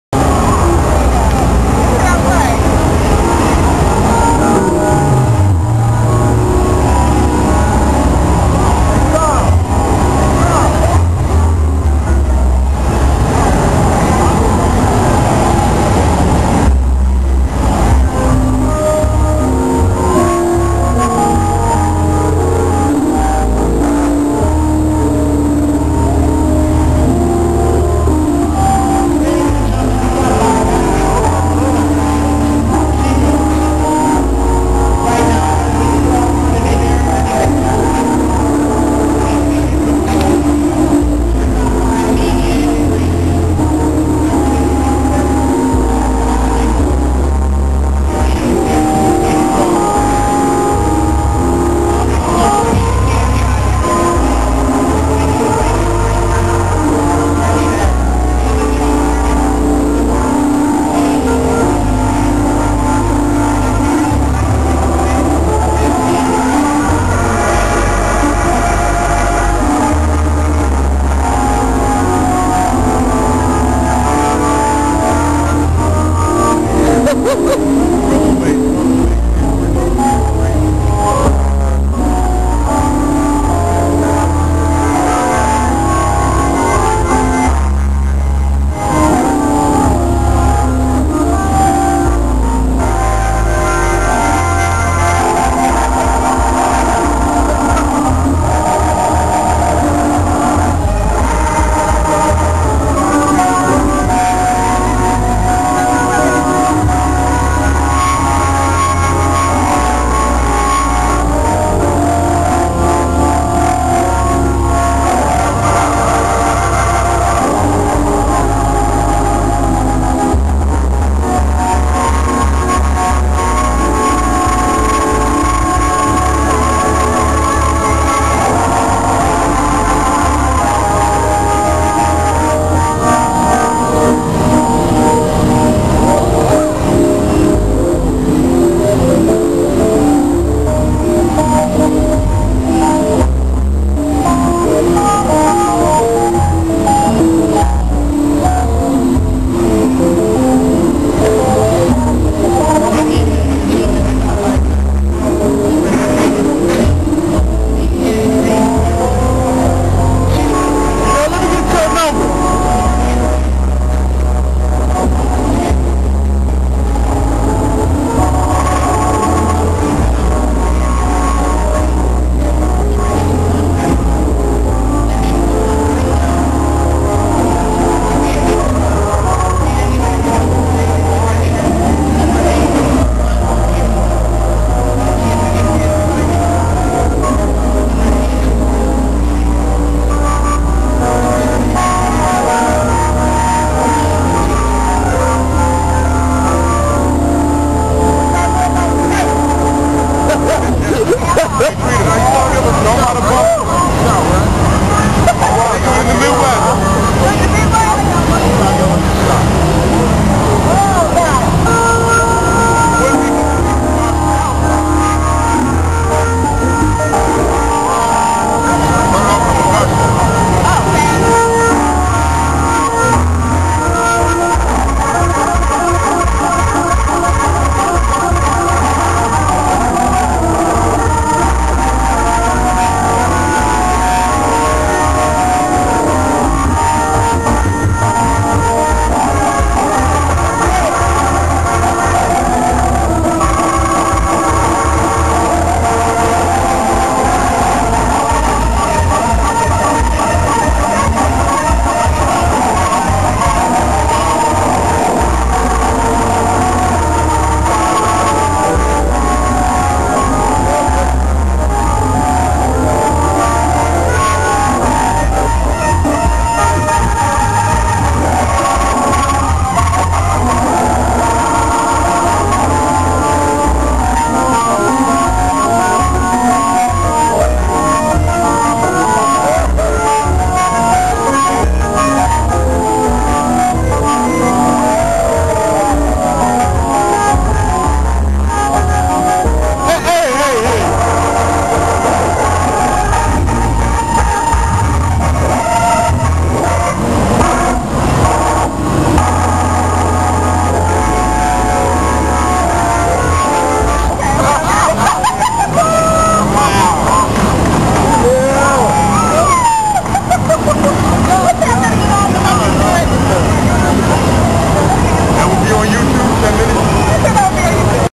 Trina Trine at GMWA 2007
Just me playing around at the DiversiOrgan with Sebastian Wheat (Wheatworks) and PJ Morgan behind me.